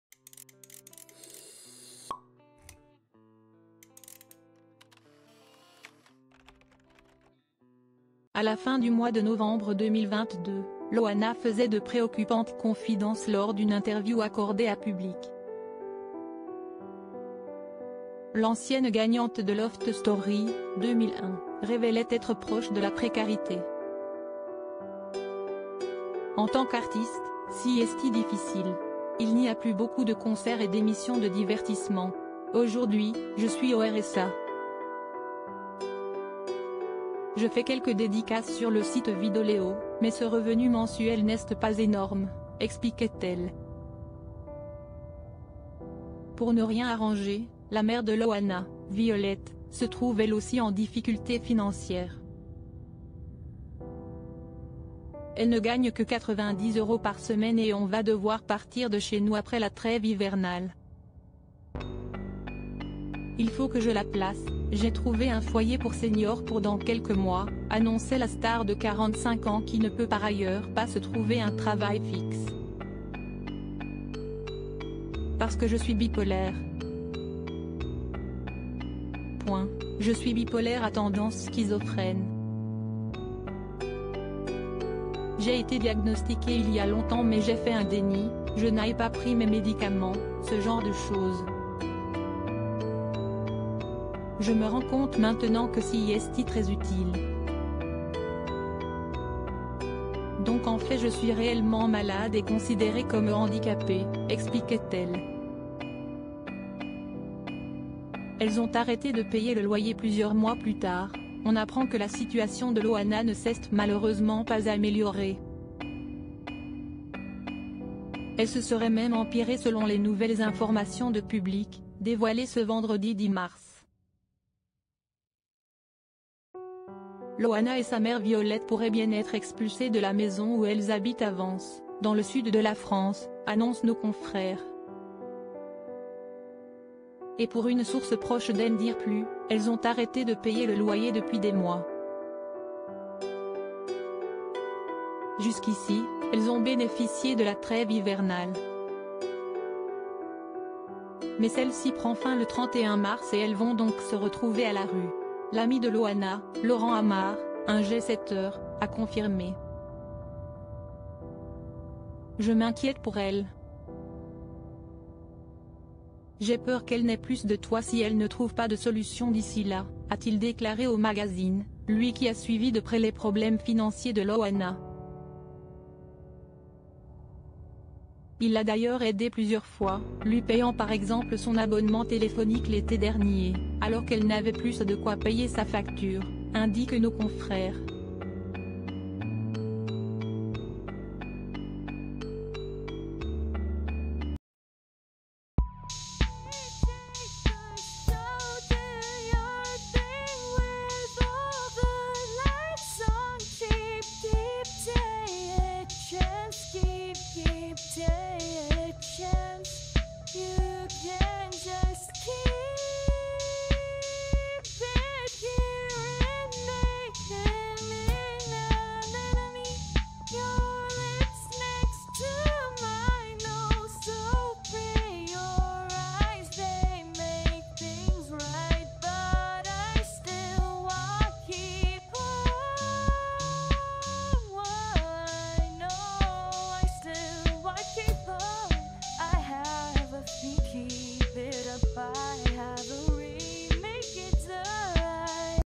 "Elles vont se retrouver à la rue !" : Loana et sa mère dans une situation critique, dernières nouve
"Elles vont se retrouver à la rue !" : Loana et sa mère dans une situation critique, dernières nouvelles préoccupantes